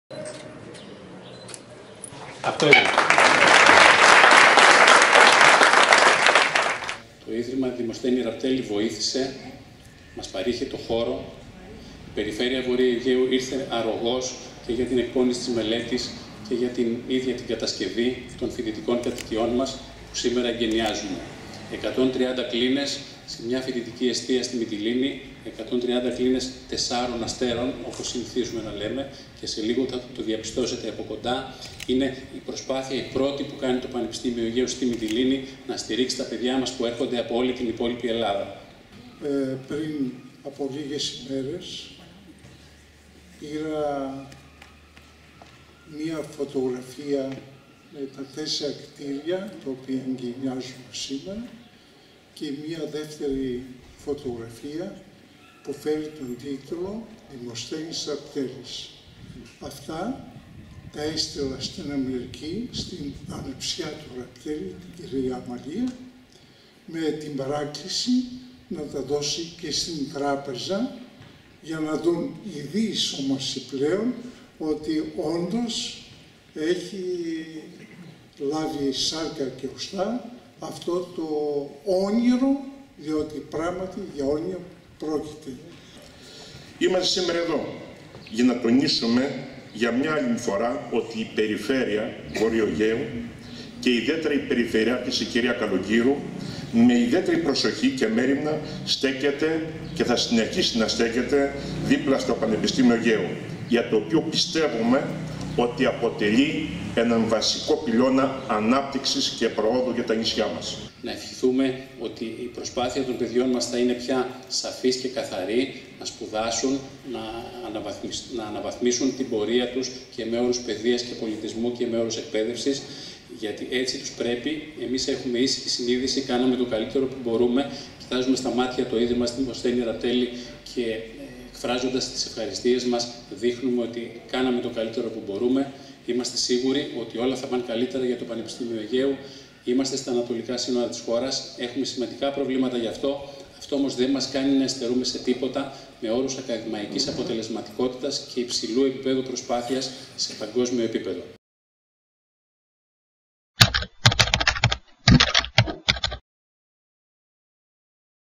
Εγκαινιάστηκαν οι φοιτητικές κατοικίες στη Μυτιλήνη
Υπερσύγχρονες και σε λειτουργία από τα τέλη του 2016, παρουσία  των πρυτάνεων του Παν. Αιγαίου των τελευταίων χρόνων